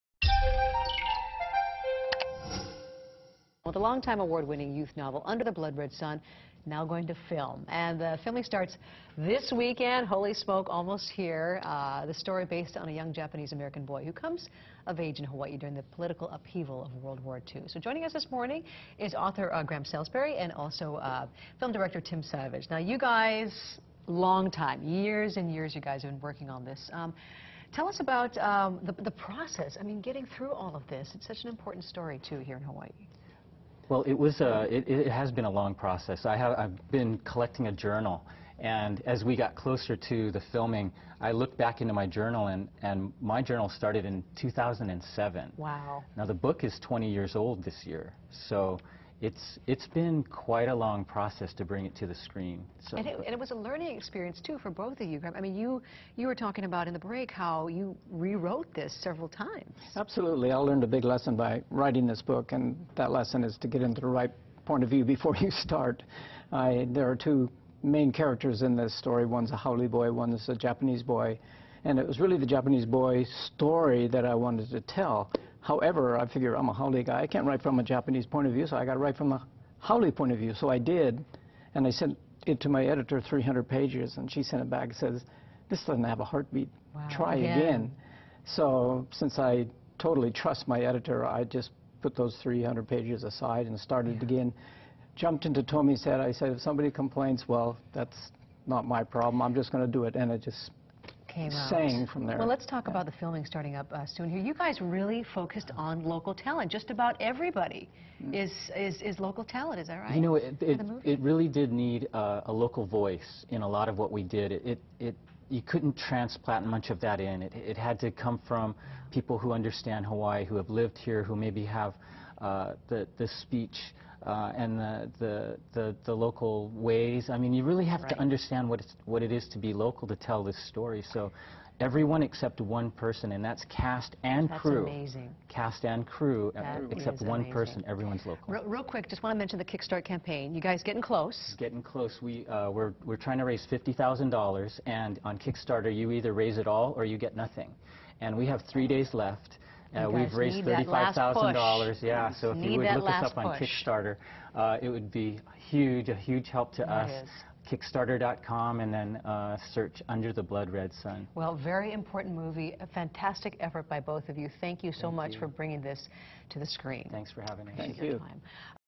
Book turned movie begins filming in Hawaii
The longtime award winning youth novel, "Under the Blood Red Sun," is now going to film! Filming starts this weekend here in Hawaii, author Granham Salisbury and fim director Tim Savage talk about the work it took to get this story to the big screen For news you need to know,